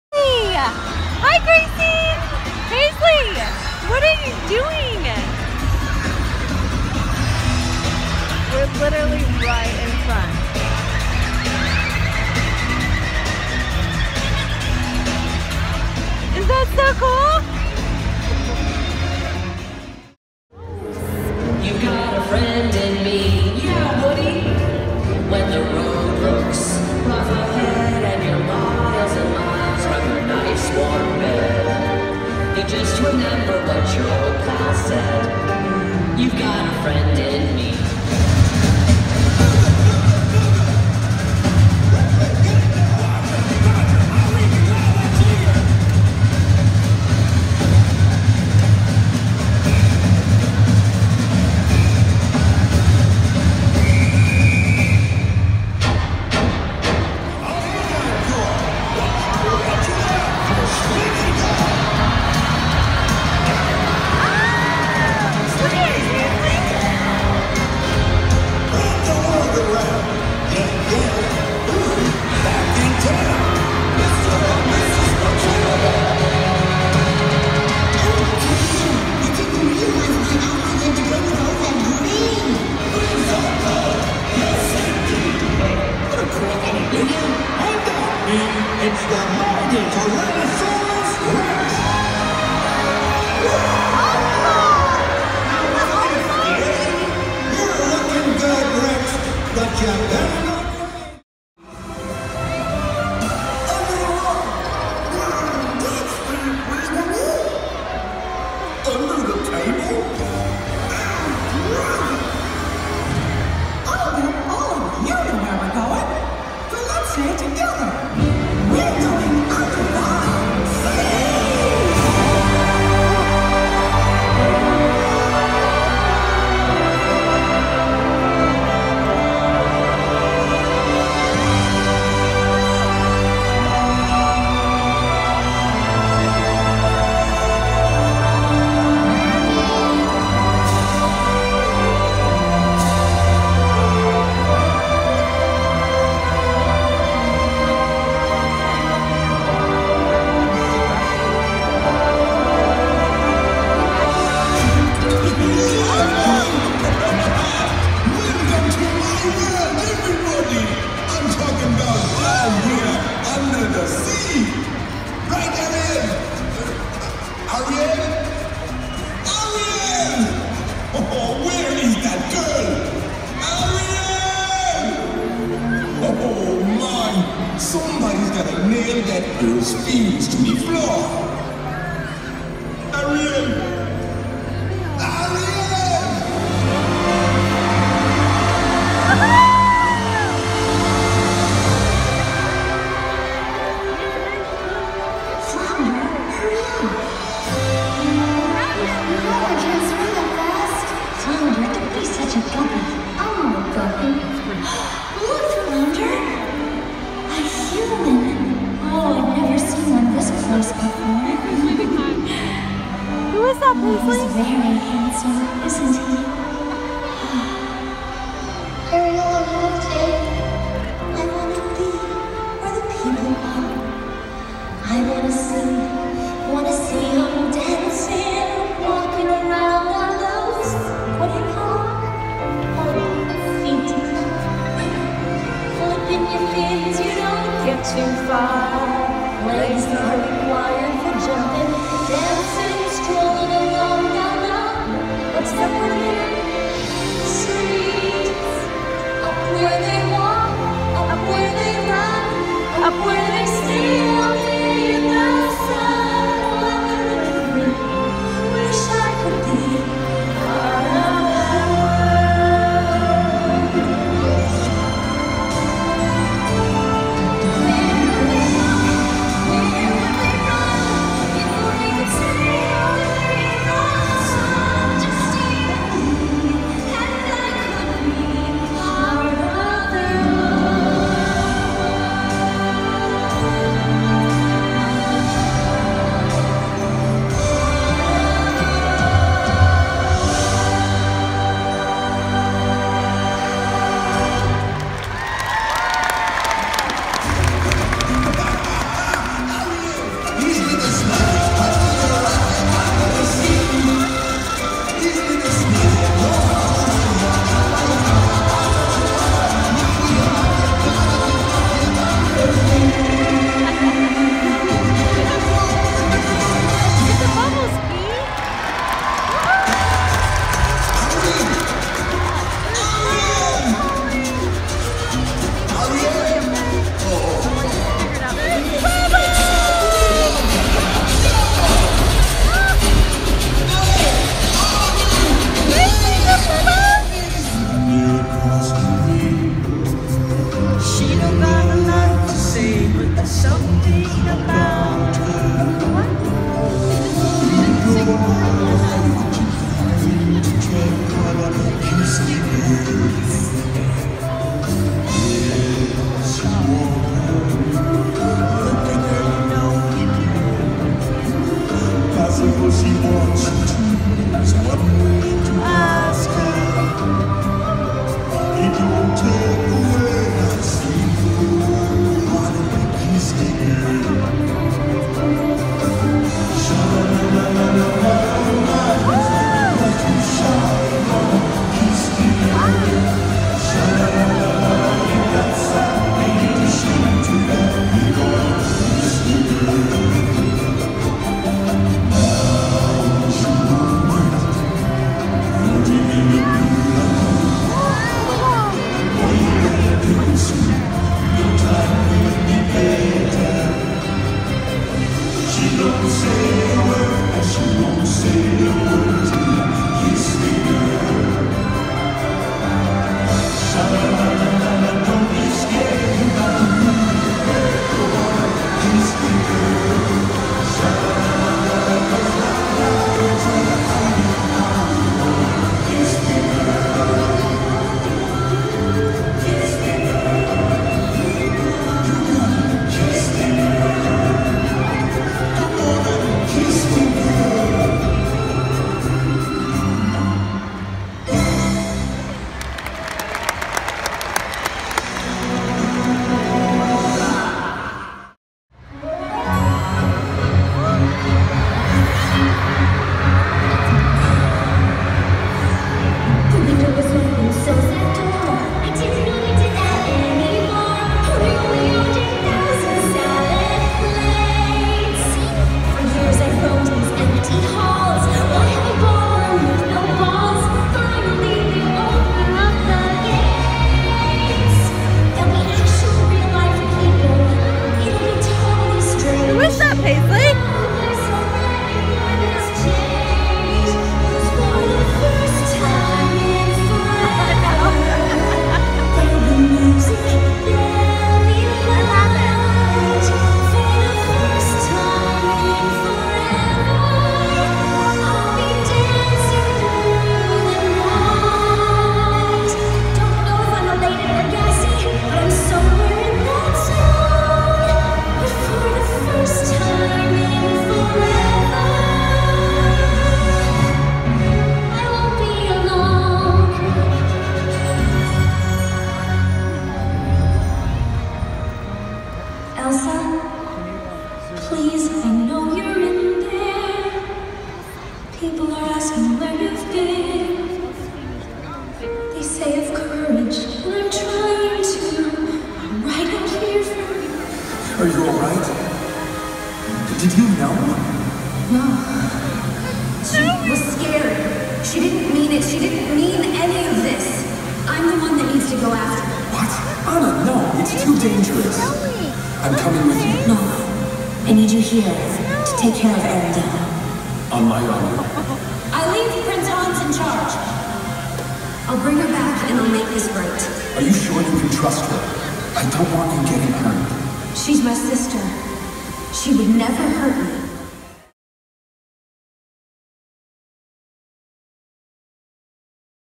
Disney On Ice "World Of Enchantment"
One of the best shows I have ever been to by far. We were blessed by literally getting front row seats. My two year old had so much fun and sat practically still the entire two hours.